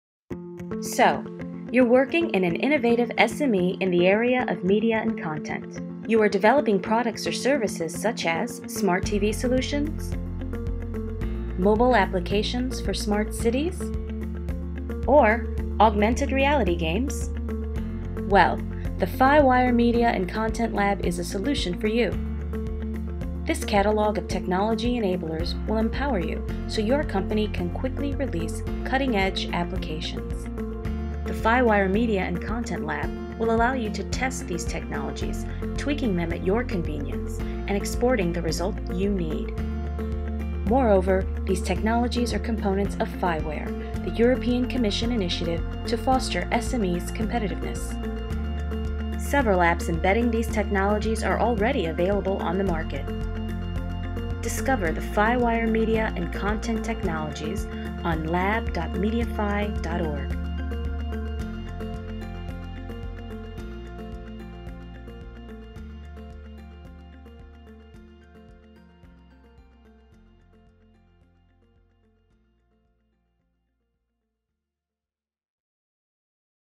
FIWARE Media & Content Lab - Introduction
A presentation of the FIWARE Media & Content Lab: the FIWARE portal to test, tweak and run FIWARE enablers for media & content.